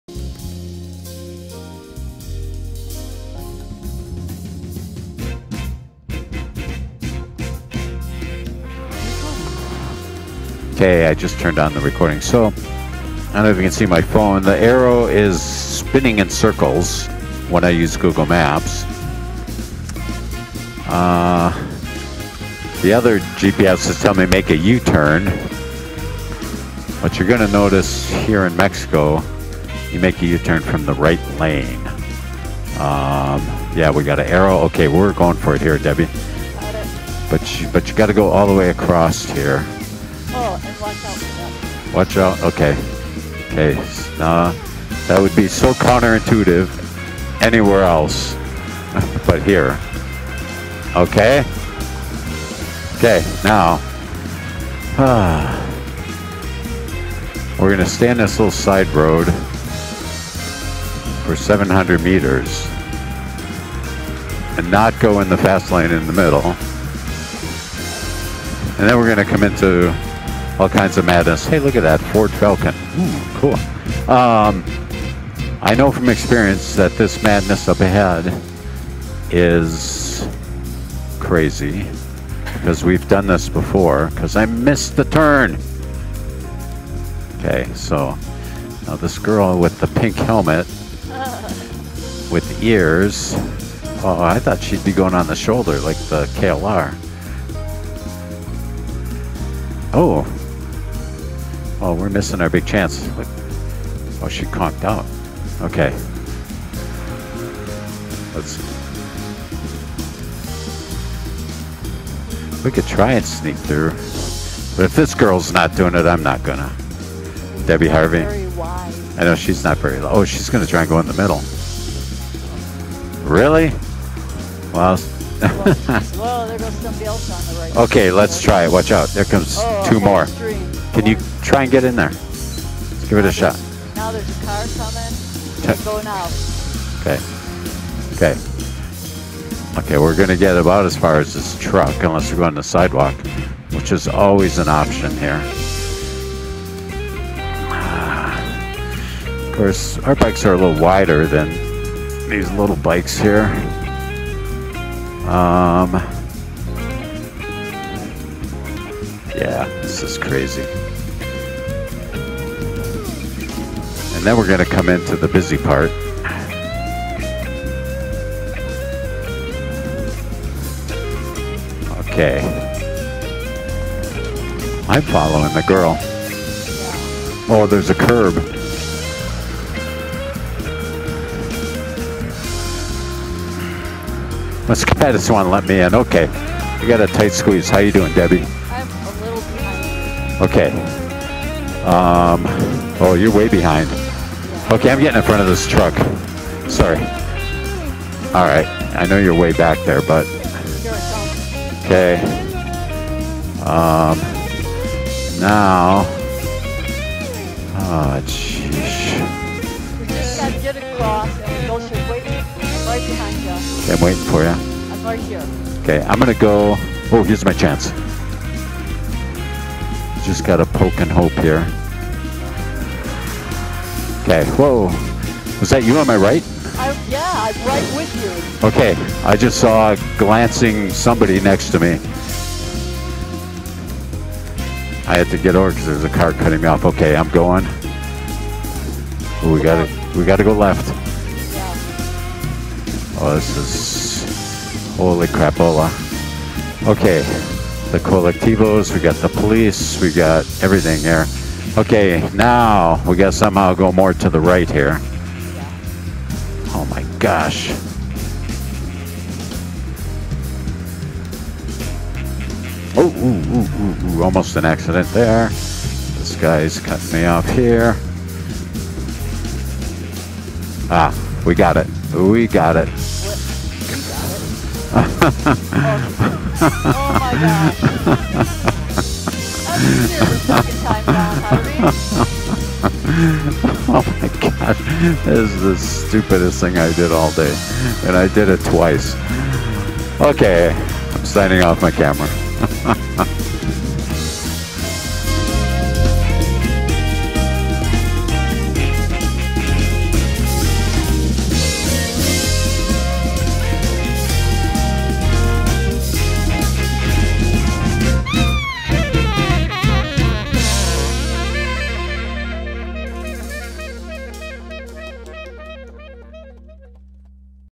Adventure Motorcycling in Morelia, Michoacán, Mexico
Sometimes adventure motorcycle riding is not just remote backroads but city driving. Live helmet communications with my wife behind me as we try to negotiate city traffic in Morelia, Michoacan, Mexico.